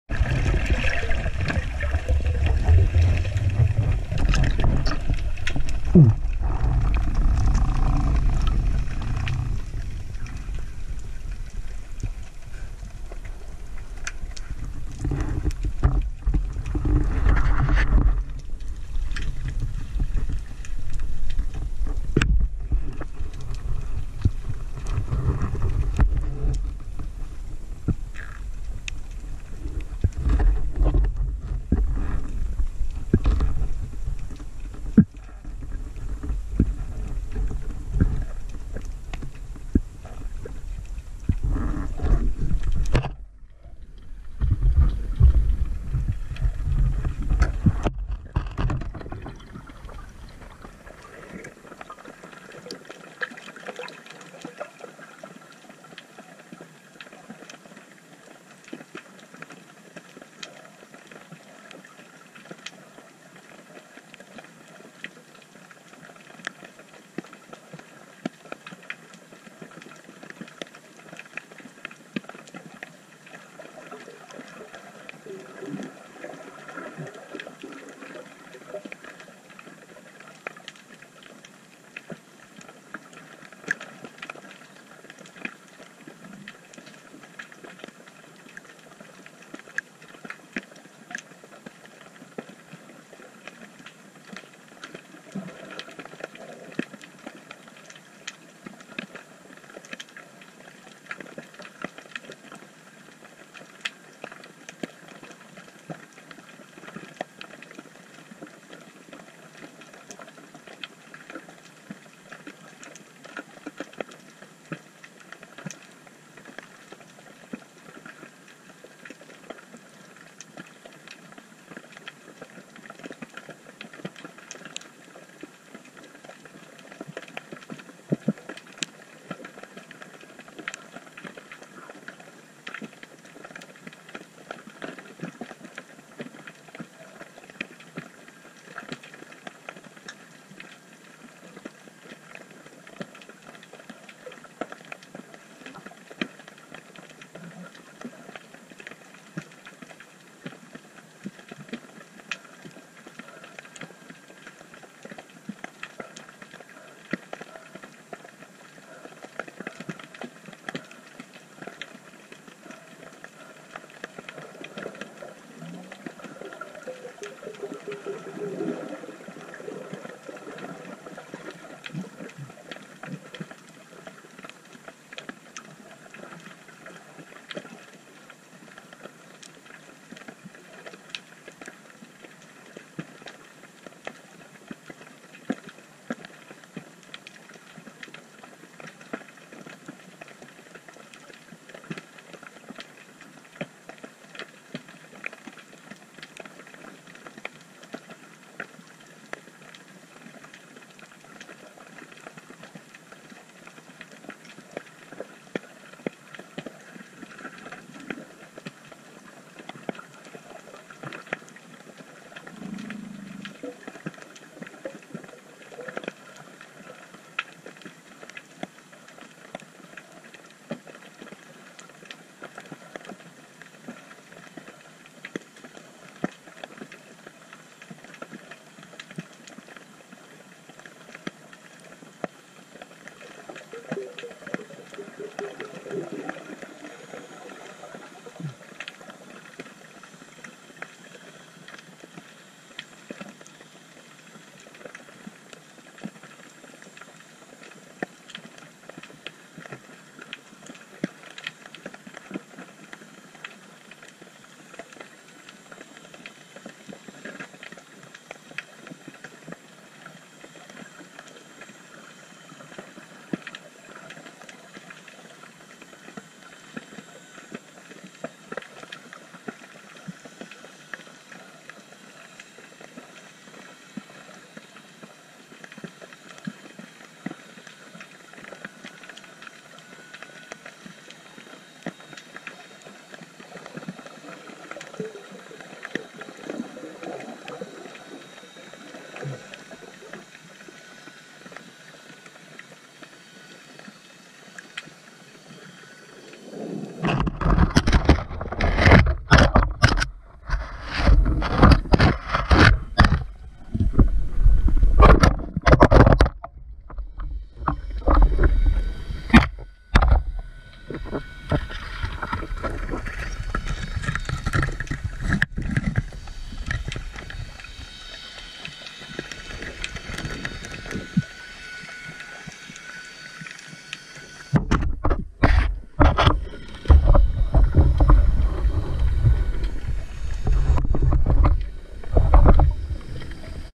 CAÇA SUB CASCAIS - SARGO DE + DE 1KG entocado
Lance completo sem cortes . Interessante para observar movimento de peixe na toca. Outubro de 2020.